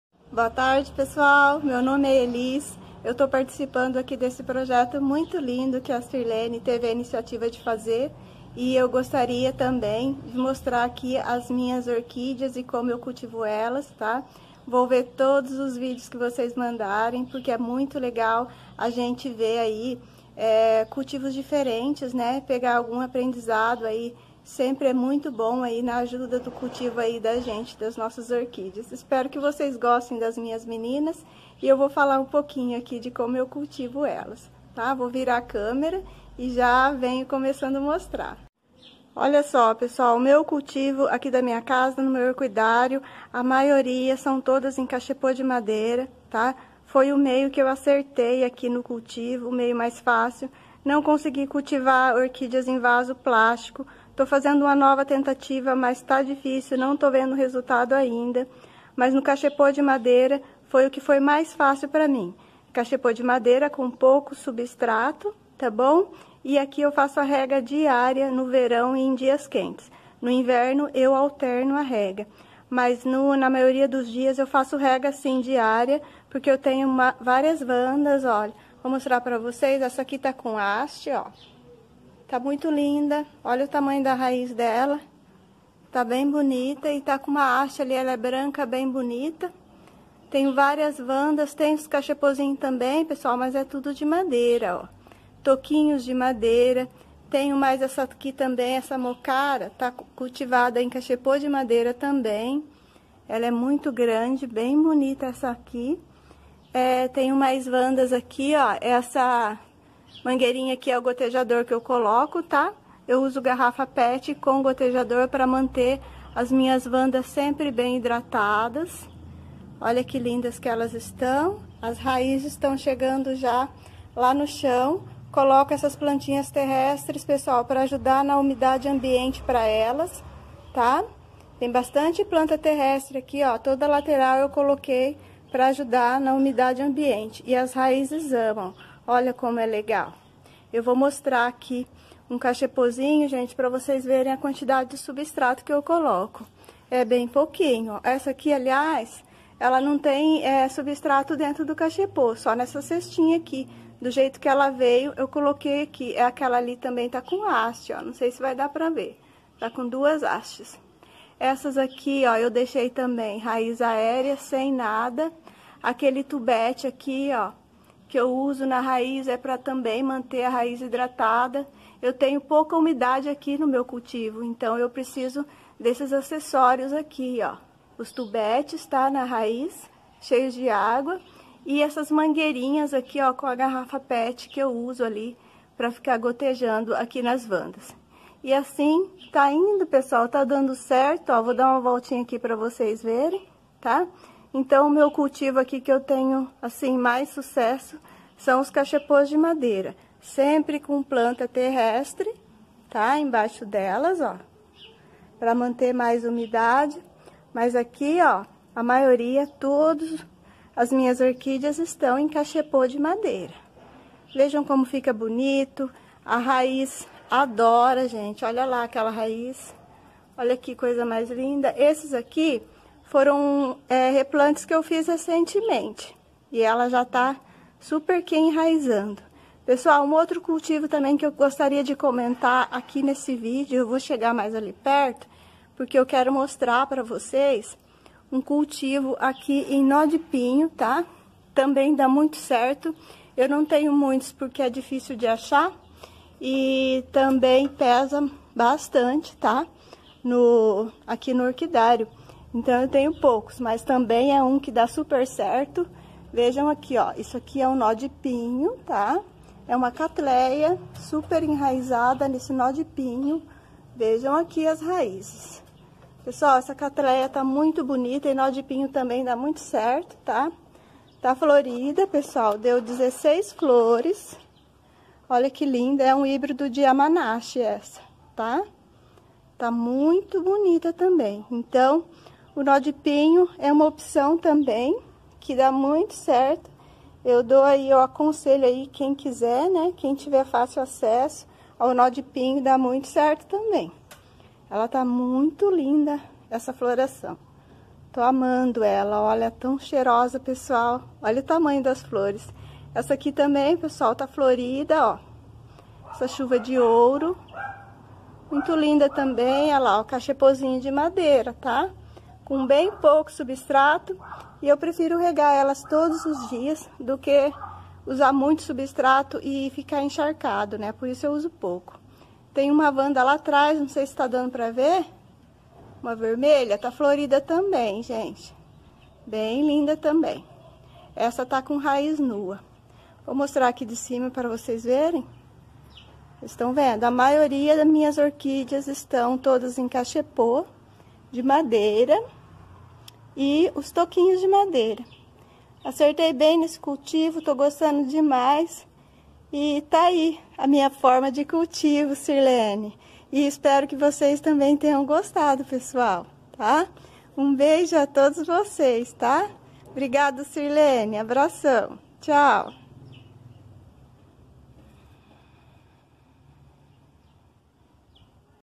Participação do projeto da amiga Sirlene! Como cultivo minhas orquídeas!
Boa tarde amigos, hoje venho mostrar mais um pouco sobre meu modo de cultivo de orquídeas, convite da amiga Sirlene, adorei esse projeto. Vamos trocar nossas experiências sobre o cultivo de orquídeas 🌹